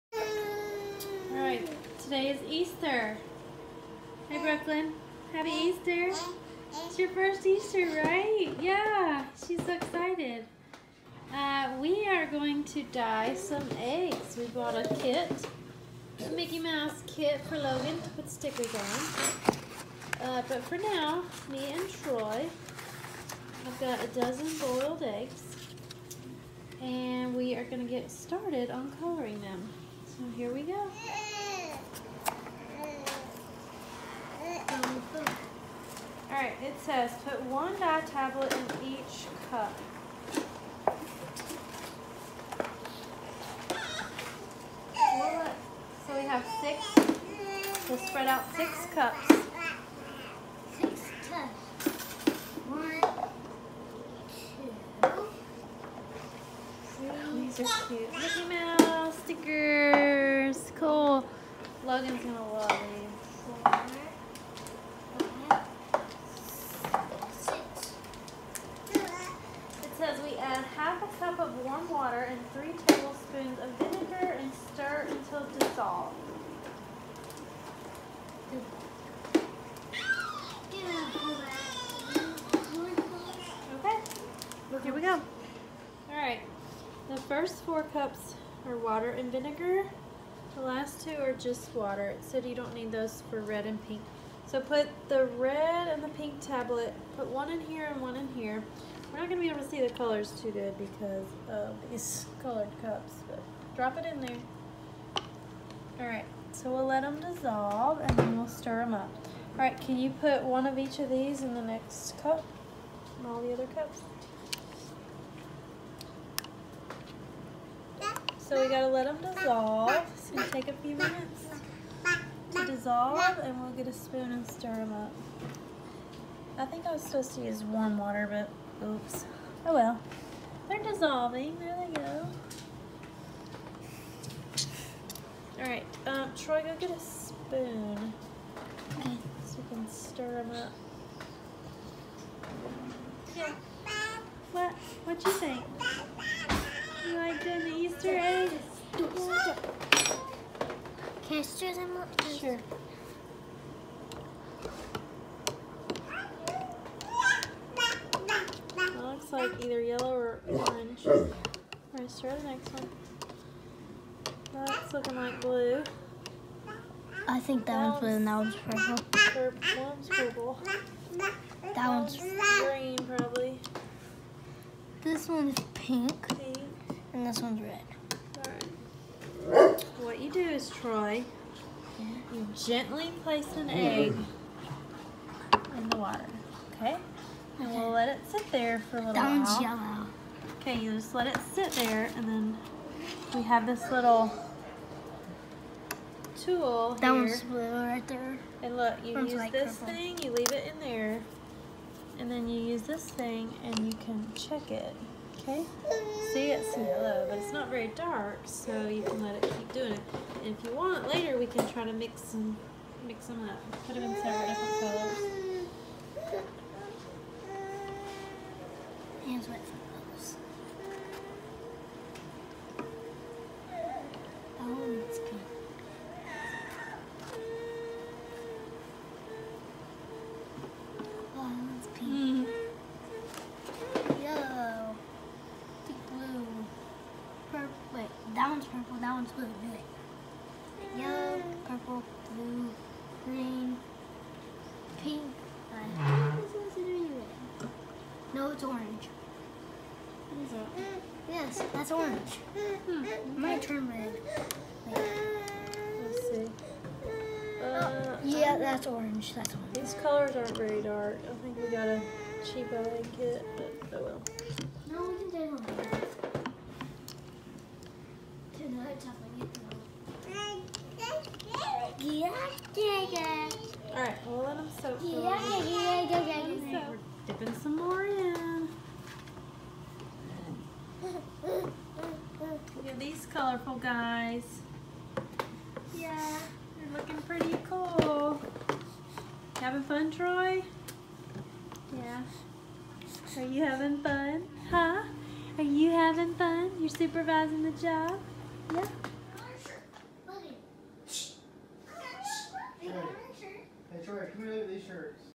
Dying Easter Eggs 2018
Troy dyed Easter eggs.